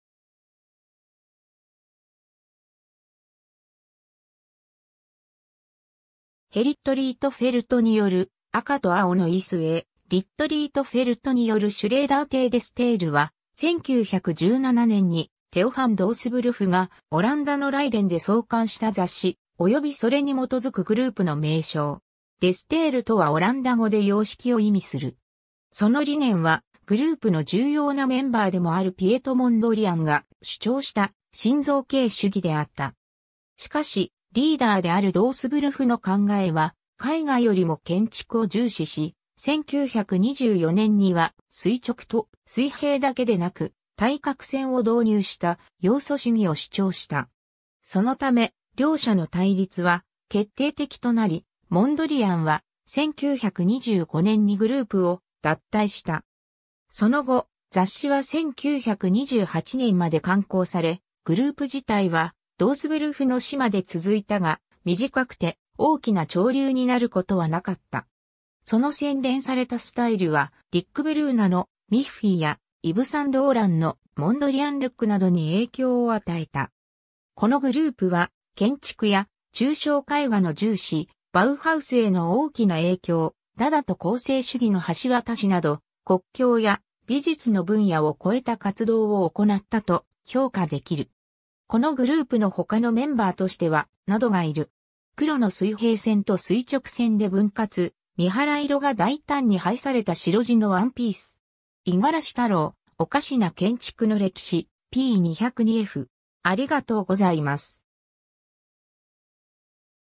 デ・ステイル
アヴァンギャルド・アート
ヘリット・リートフェルトによる「赤と青のいす」(1917) ヘリット・リートフェルトによるシュレーダー邸 デ・ステイル (De Stijl) は、1917年にテオ・ファン・ドースブルフ（英: Theo van Doesburg, 1883年 - 1931年）がオランダのライデンで創刊した雑誌、及びそれに基づくグループの名称。
「デ・ステイル」とはオランダ語で様式（英語：The Style）を意味する。
 その理念は、グループの重要なメンバーでもあるピエト・モンドリアンが主張した新造形主義（ネオ・プラスティシズム、英: Neoplasticism、蘭: Nieuwe beelding）であった。
しかし、リーダーであるドースブルフの考えは、絵画よりも建築を重視し、1924年には、垂直と水平だけでなく、対角線を導入した要素主義（エレメンタリズム）を主張した。
そのため、両者の対立は決定的となり、モンドリアンは、1925年にグループを脱退した。
 その後、雑誌は1928年まで刊行され、グループ自体はドースブルフの死（1931年）まで続いたが、短くて大きな潮流になることはなかった。
その洗練されたスタイルはディック・ブルーナの「ミッフィー」（1955年）や、イヴ・サン＝ローランの「モンドリアン・ルック」（1955年）などに影響を与えた。
 このグループは、建築や抽象絵画の重視、バウハウスへの大きな影響、ダダと構成主義の橋渡しなど、国境や美術の分野を越えた活動を行ったと評価できる。
 このグループの他のメンバーとしては、 などがいる。
 ^ 黒の水平線と垂直線で分割、三原色が大胆に配された白地のワンピース。
 ^ 五十嵐太郎『おかしな建築の歴史』（エクスナレッジ、2013年）p.202f.